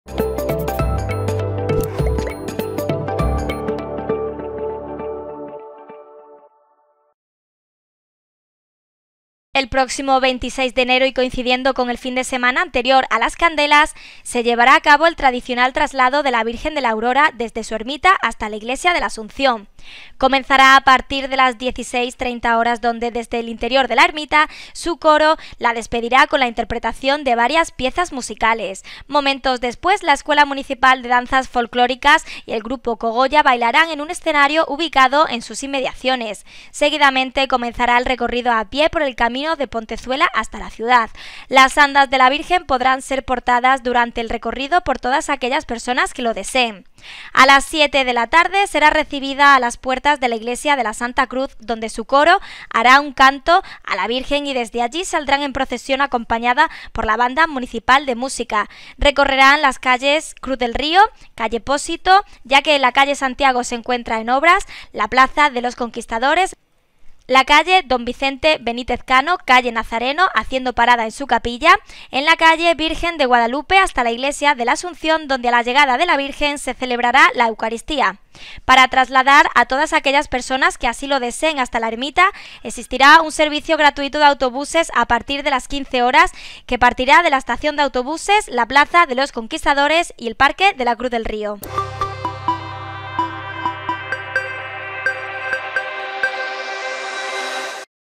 La traída de la Virgen de la Aurora a la ciudad será el 26 de enero.
La traída de la Virgen de la Aurora a la ciudad será el 26 de enero. Con autobuses gratuitos en distintos puntos de la localidad.

El próximo 26 de enero y coincidiendo con el fin de semana anterior a Las Candelas, se llevará a cabo el tradicional traslado de la Virgen de la Aurora desde su ermita hasta la Iglesia de Nuestra Señora de la Asunción. Comenzará a partir de las 16:30 horas donde desde el interior de la ermita, el coro de la Hermandad de la Virgen de la Aurora la despedirá con la interpretación de varias piezas musicales. Momentos después, la Escuela Municipal de Danzas Folklóricas de Villanueva de la Serena y el grupo Cogolla, bailarán en un escenario ubicado en sus inmediaciones. Seguidamente comenzará el recorrido a pie por el camino de Pontezuela hasta la ciudad. Las andas de la Virgen podrán ser portadas durante el recorrido por todas aquellas personas que lo deseen.